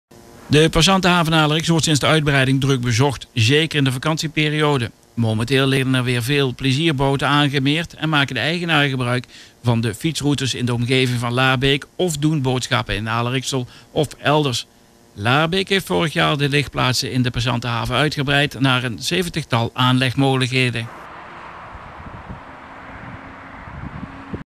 2015 07 28 Omroep Kontakt Laarbeek 2015 07 27 Passantenhaven Aarle Rixtel
De Passantenhaven in Aarle-Rixtel wordt sinds de uitbreiding druk bezocht. Zeker in de vakantieperiode. Momenteel liggen er weer veel plezierboten aangemeerd en maken de eigenaren gebruik van de fietsroutes in de omgeving van Laarbeek of doen boodschappen in Aarle-Rixtel of elders. Laarbeek heeft vorig jaar de ligplaatsen in de passantenhaven uitgebreid naar  een zeventigtal  aanlegmogelijkheden.